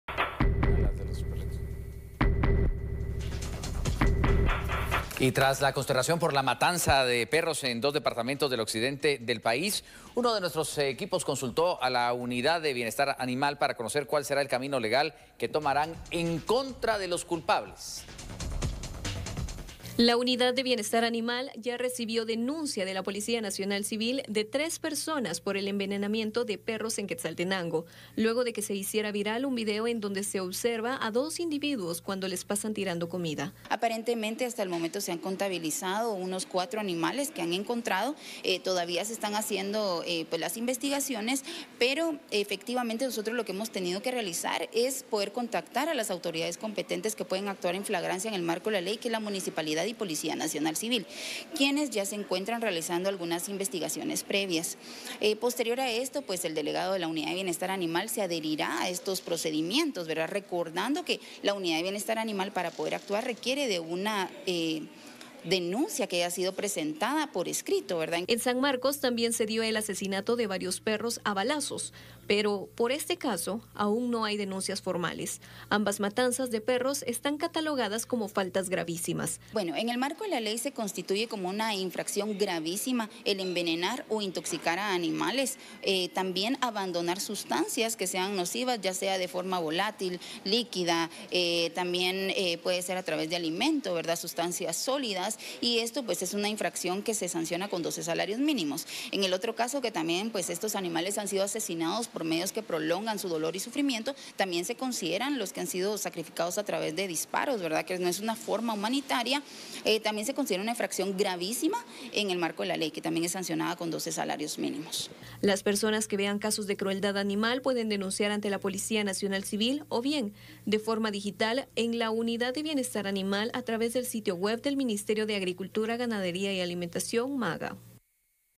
Unidad de Bienestar Animal: El envenenamiento de perros es una falta gravísima
Tras la consternación por la  matanza de perros en dos departamentos del occidente del país, Noti 7 consultó a la Unidad de Bienestar Animal (UBA) para conocer el camino legal que tomaran  contra los culpables.
La PNC denunció a 3 personas ante la UBA por el envenenamiento de perros en Quetzaltenango.